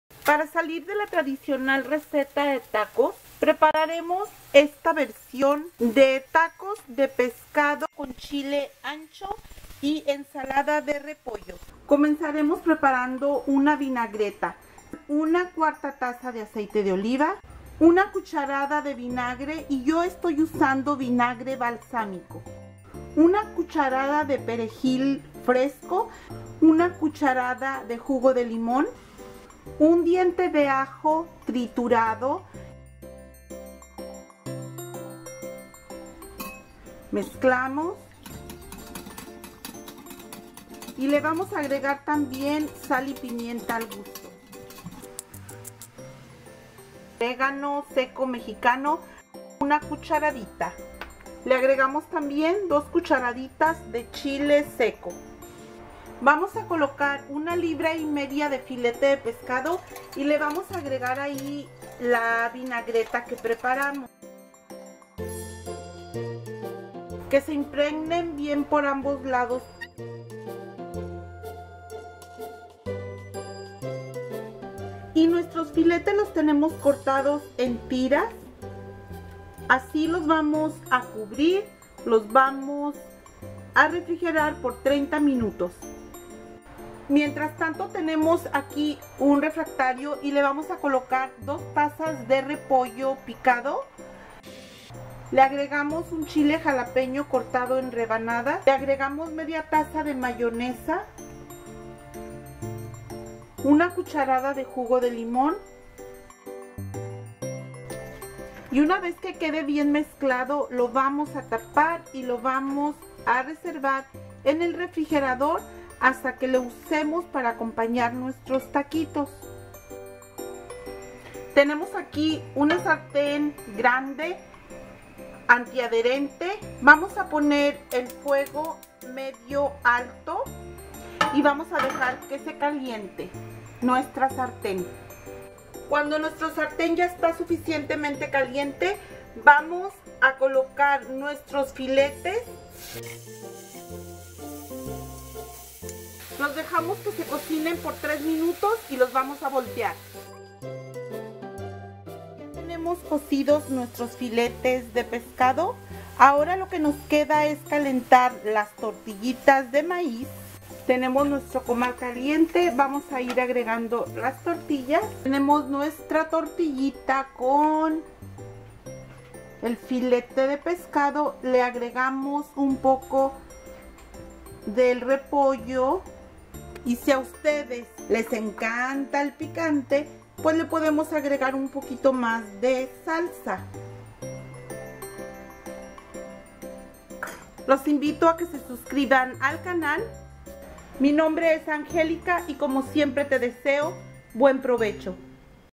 TACOS DE PESCADO CON CHILE ROJO Y ENSALADA DE REPOLLO
Para salir de la tradicional receta de tacos, prepararemos estos deliciosos TACOS DE PESCADO CON CHILE ROJO Y ENSALADA DE REPOLLO.